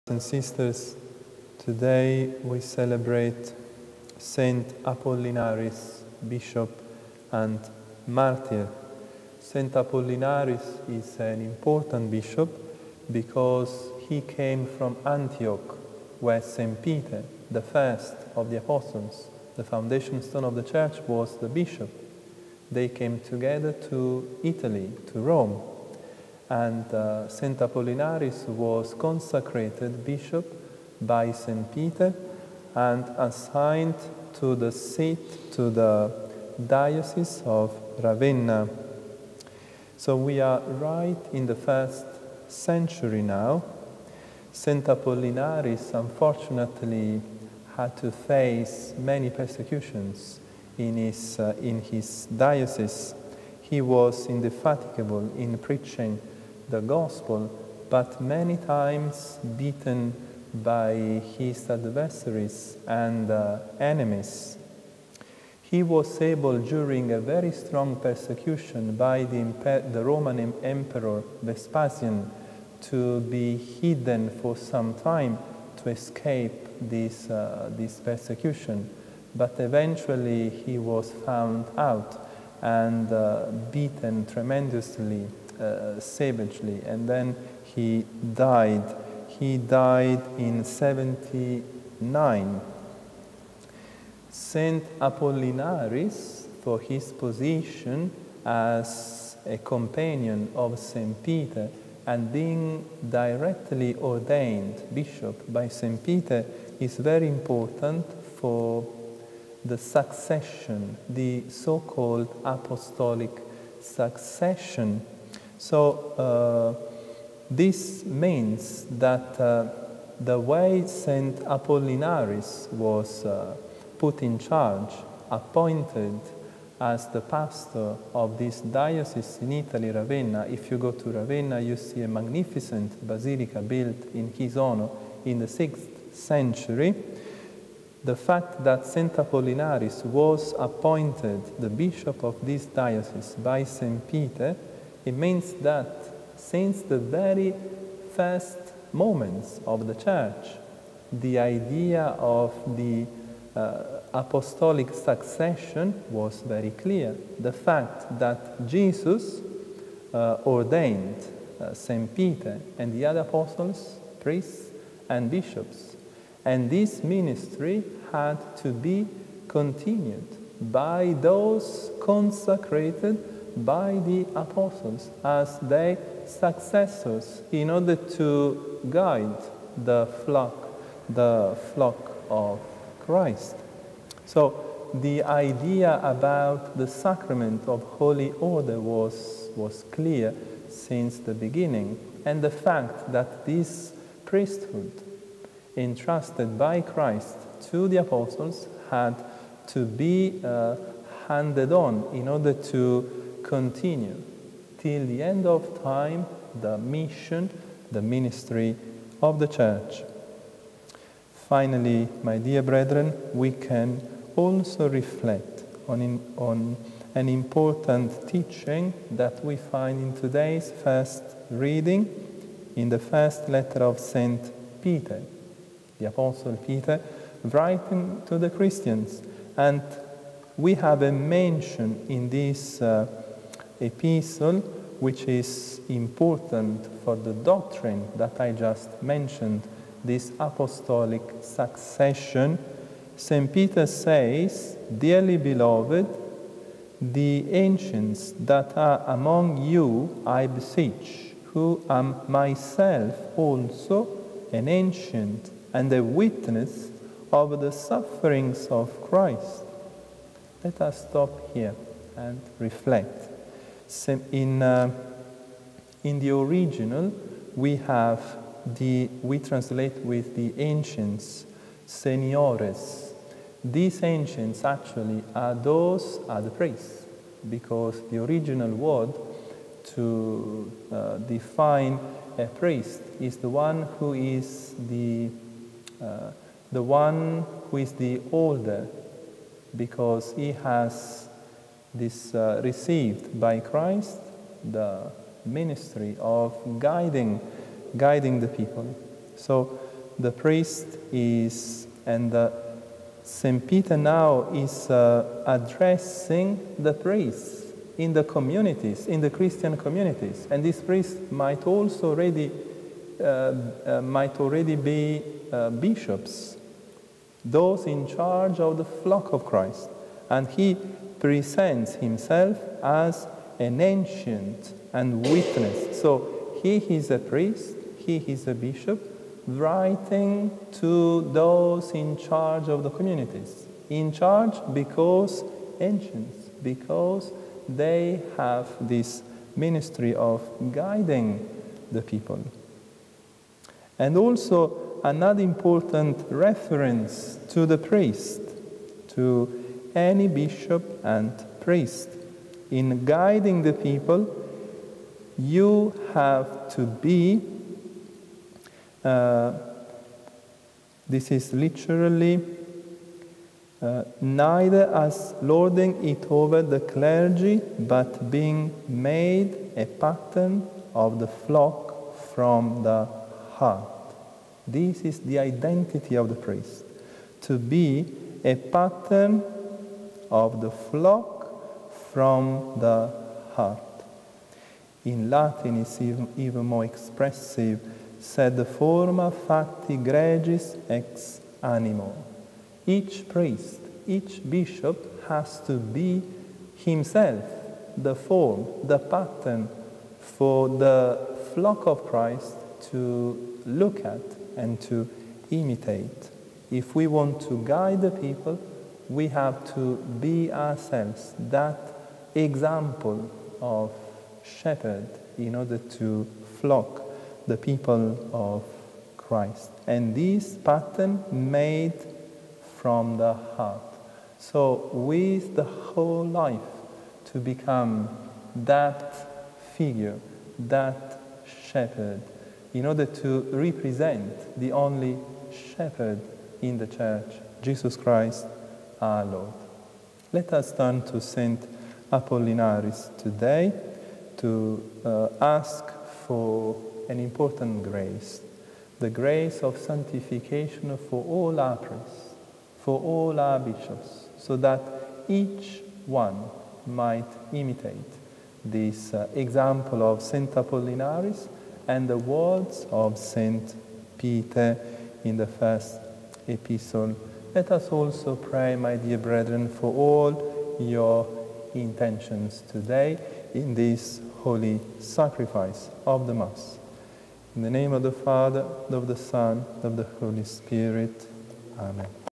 Homily Feast of St Apollinaris
According to tradition, Saint Peter sent Apollinaris to Ravenna, Italy, as its first bishop. His preaching of the Good News was so successful that the pagans there beat him and drove him from the city.